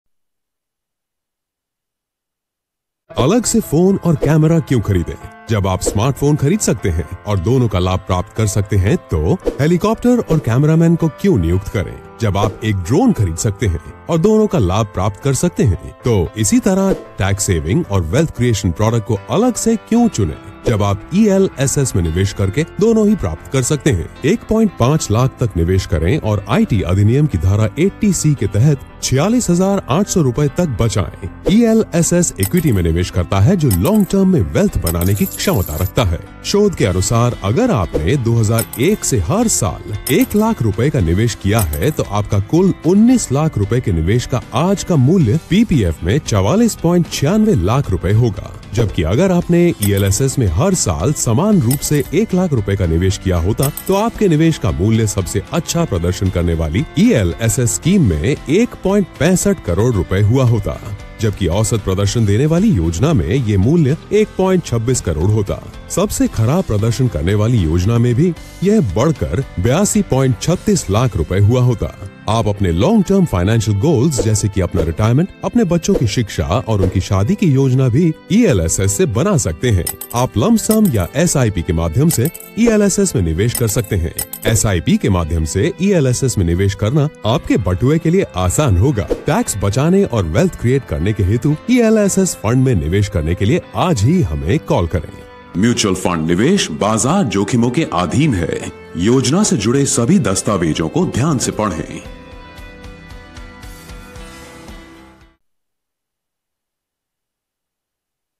ELSS Jan 2020 H
New video launched on ELSS. Video explains about save tax and create wealth -with these double benefits, one should invest in ELSS